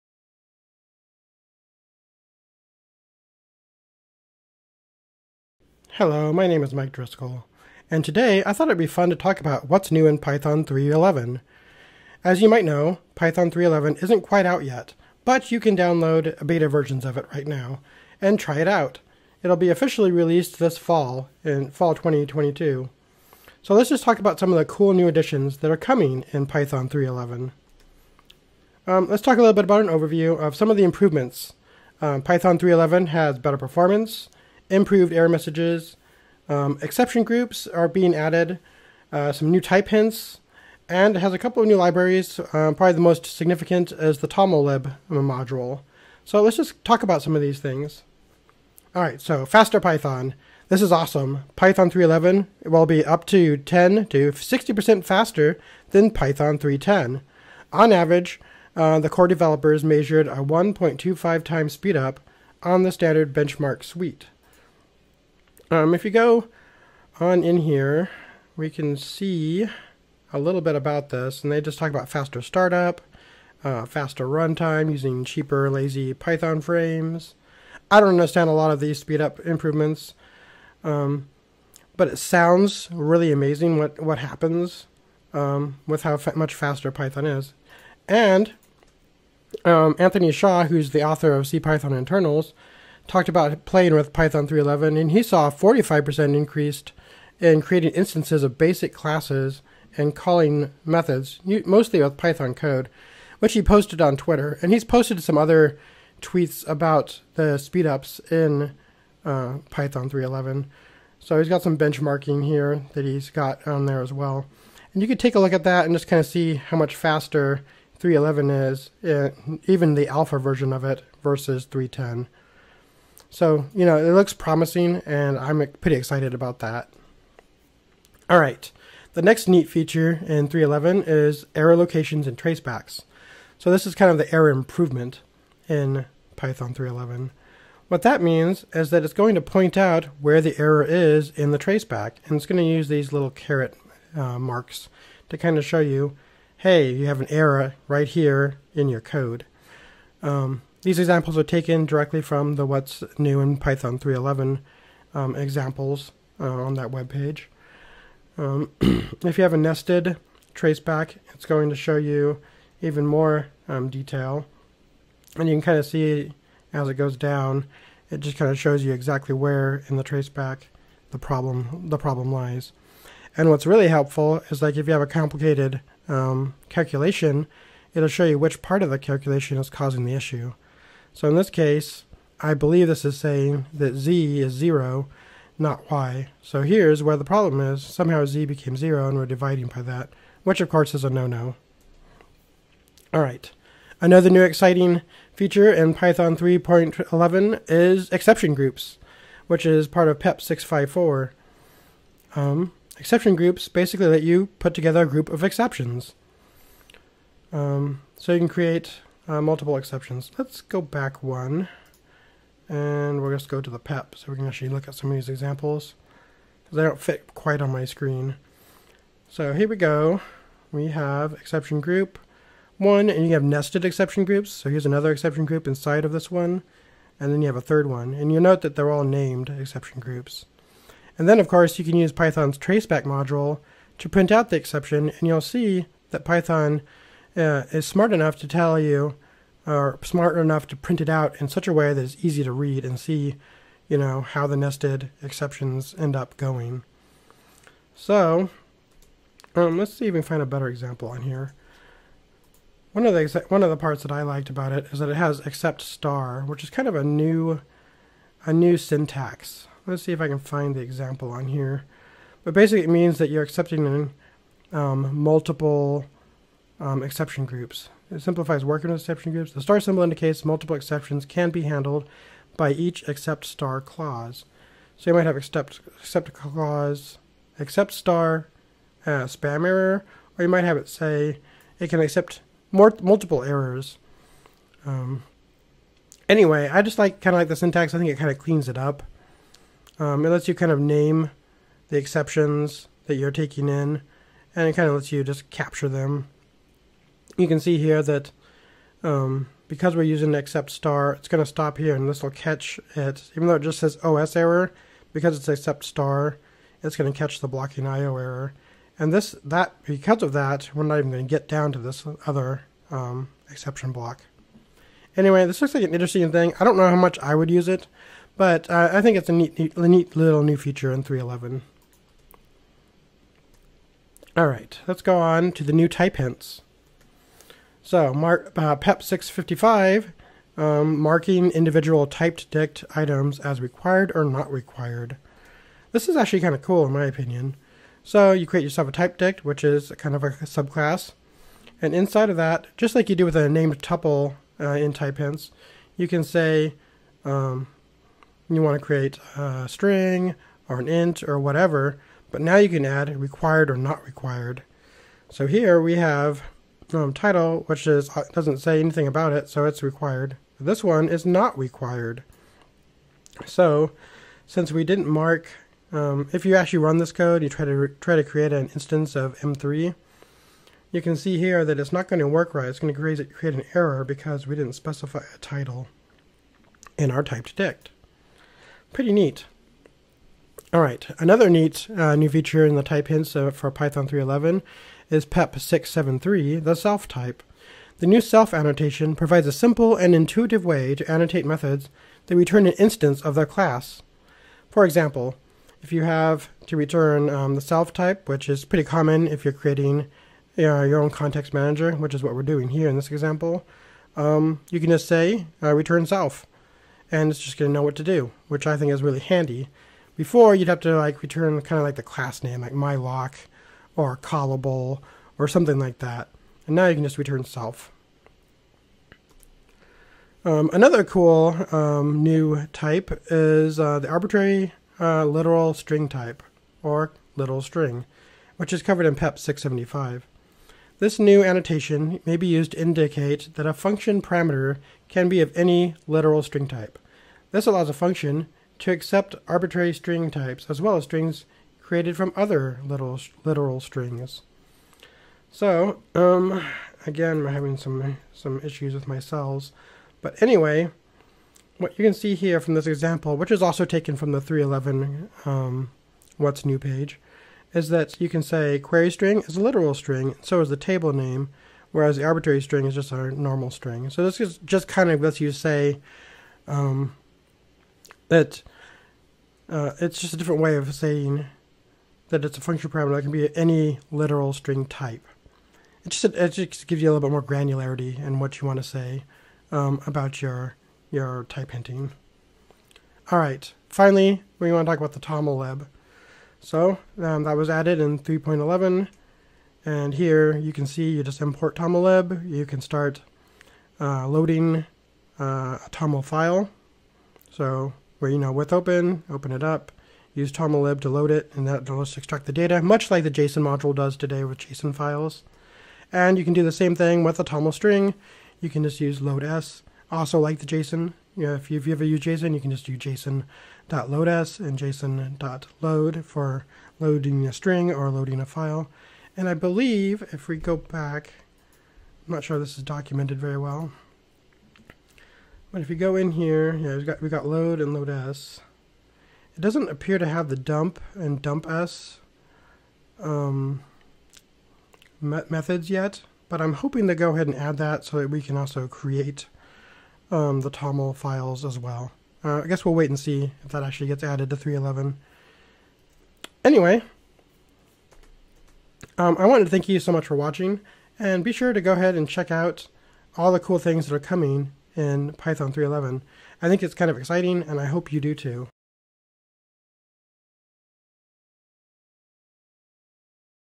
What's New in Python 3.11
In this video, Mike Driscoll talks about what to expect in Python's newest release, which is coming in Fall 2022

- Better Performance

- Improved error messages

- Exception groups

- New type hints

- The new `tomllib` module